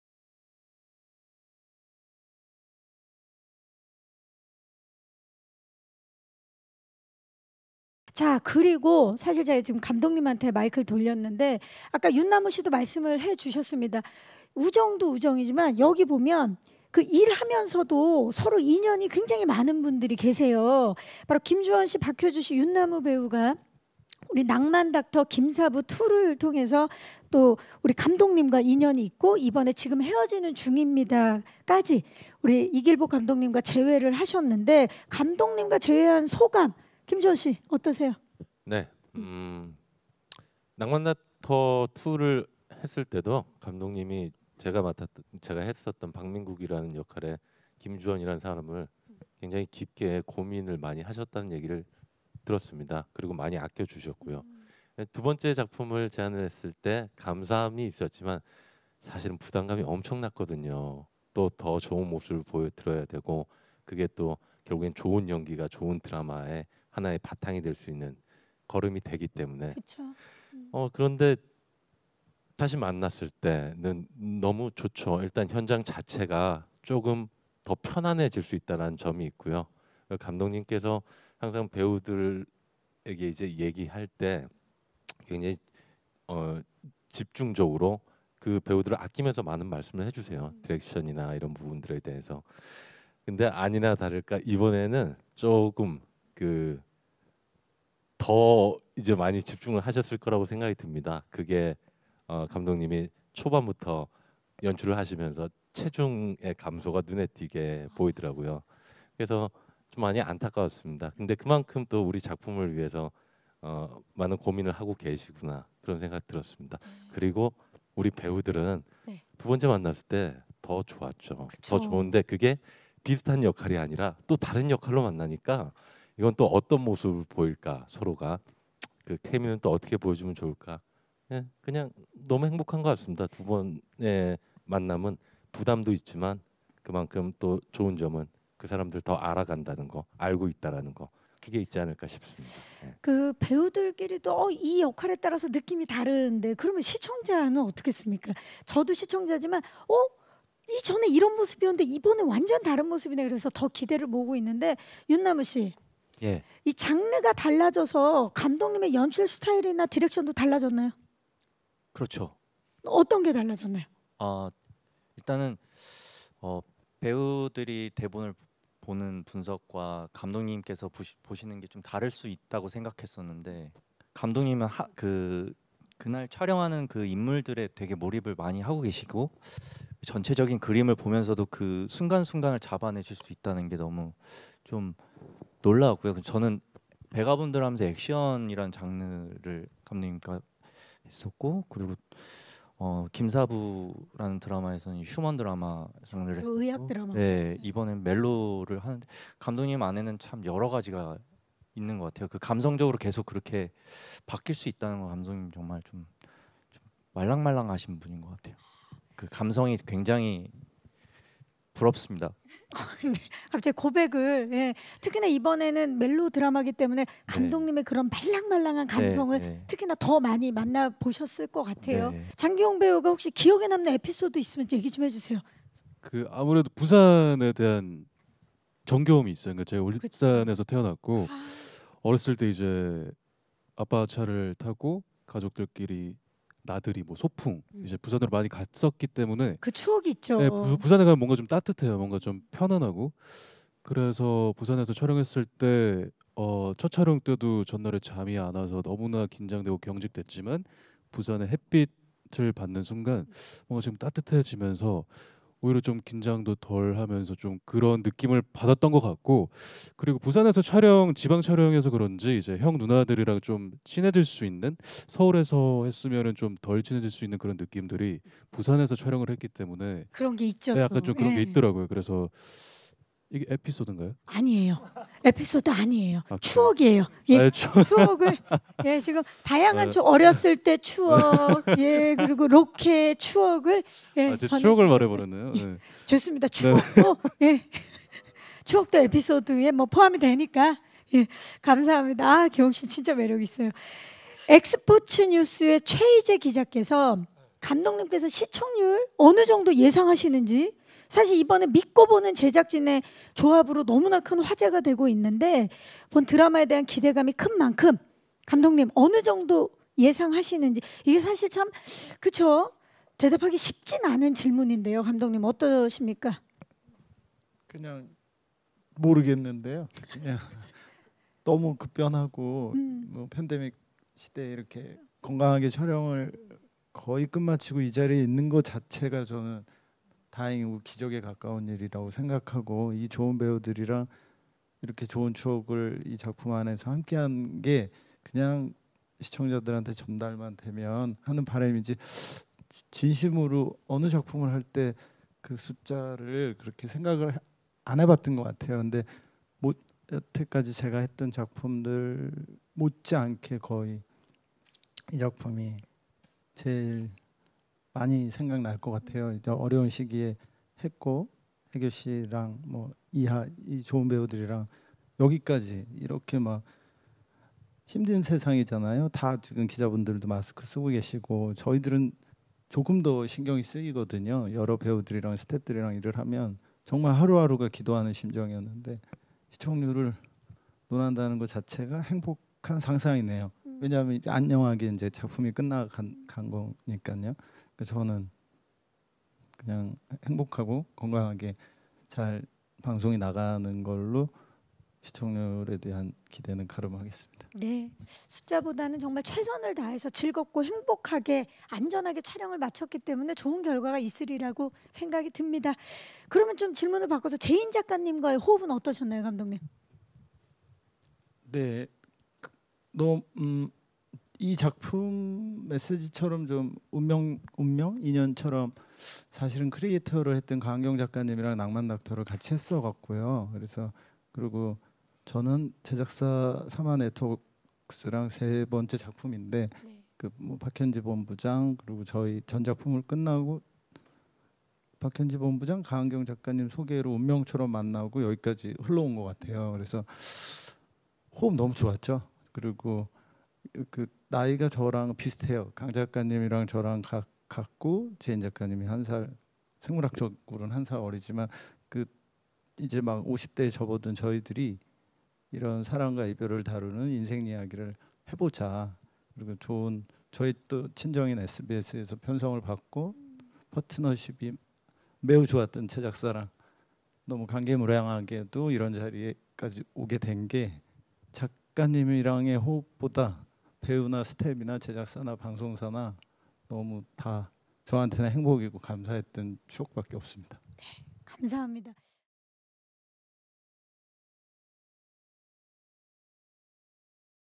Wawancara Eksklusif Pemain Viu Original Now, We Are Breaking Up Part. 7 | Viu Indonesia
Drama Viu Original Now, We Are Breaking Up memang sudah bikin para pencinta drakor penasaran sejak trailer dan teaser drama pertama dirilis. Hal ini gak dapat dipungkiri karena kehadiran para aktor dan aktris berkelas yang menghiasi drama ini 🤗 Yuk, kenalan dulu sama pemeran dalam drama Viu Original Now, We Are Breaking Up!

Saksikan Viu Original Now, We Are Breaking Up hanya di Viu